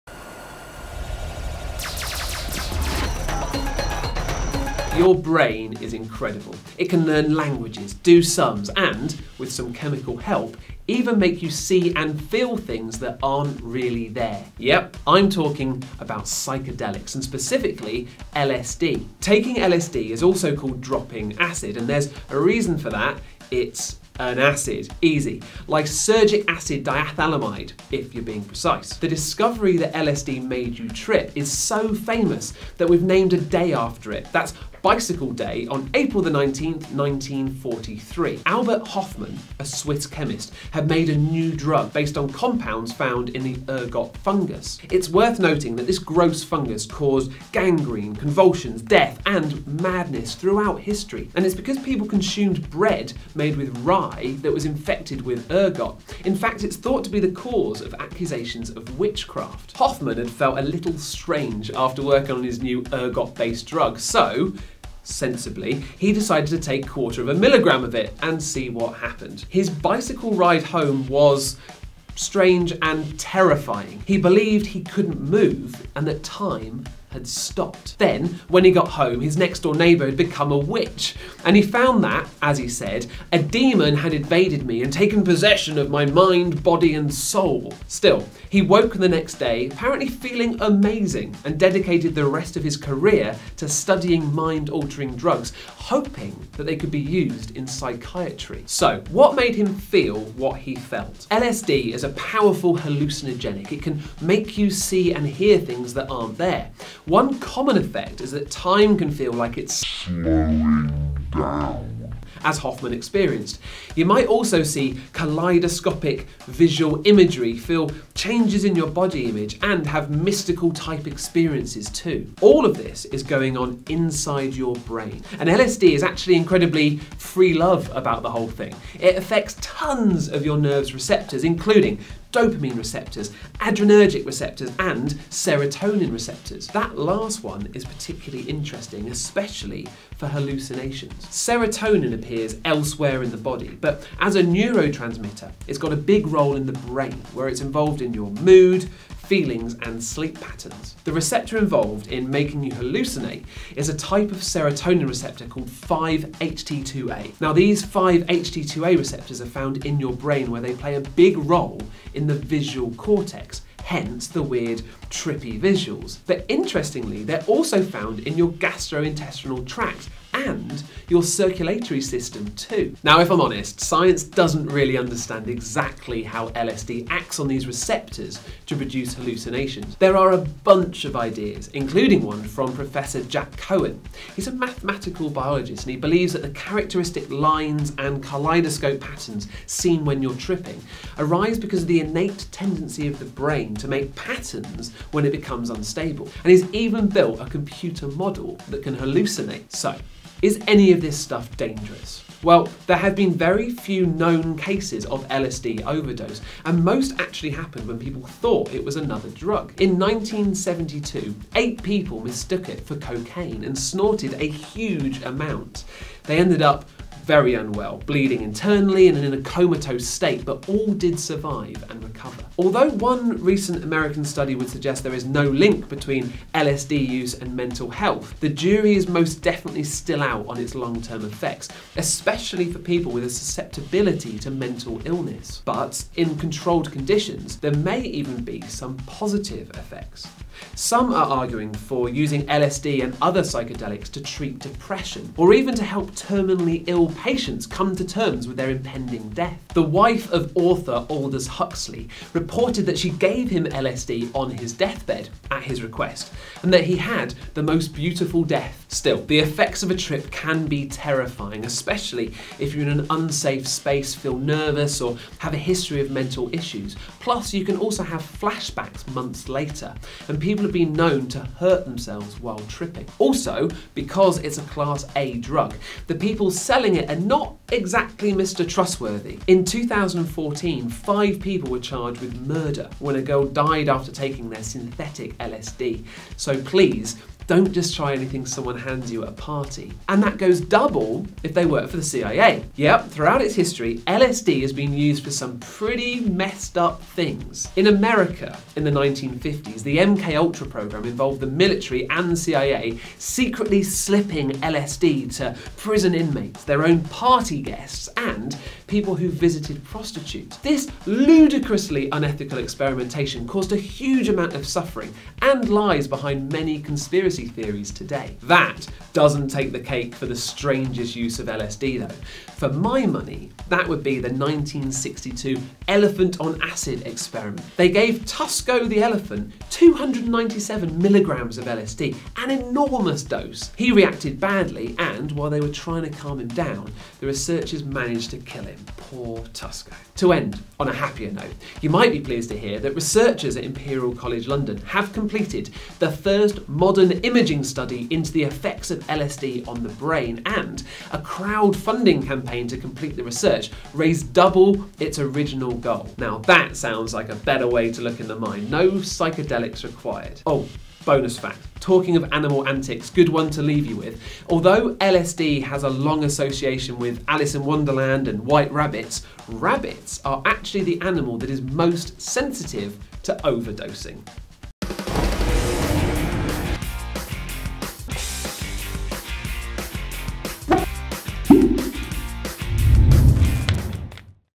What Does LSD Do To Your Brain? | Earth Science
Greg Foot explores how LSD can affect your brain. Suffice to say, don't try this at home folks.